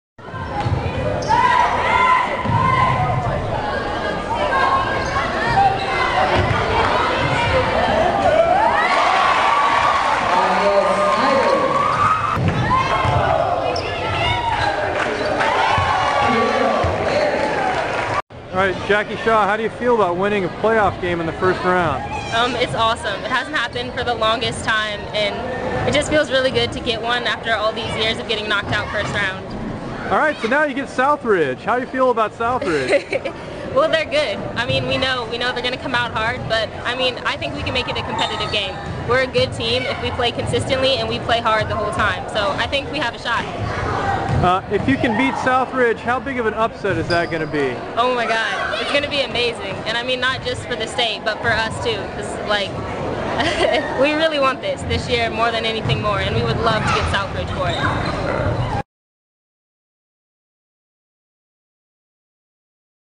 Grant preps for Southridge
Grant senior Jacqueline Shaw talks about the joy of winning a playoff game and about the upcoming duel with No. 2 Southridge Friday. Produced by Cliff Pfenning, oregonsports.com.